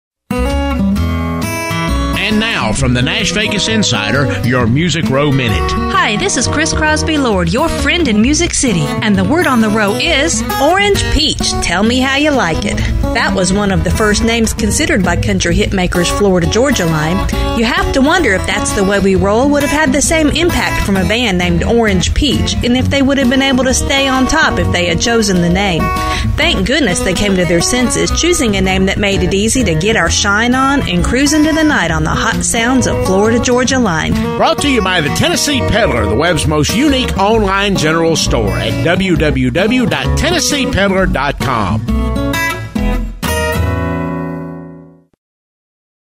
Music Row Minute 9.9.14 FL GA Line (made with Spreaker)
Was Florida Georgia Line almost called Orange Peach? Find out in todays Music Row Minute with Kris Crosby Lord.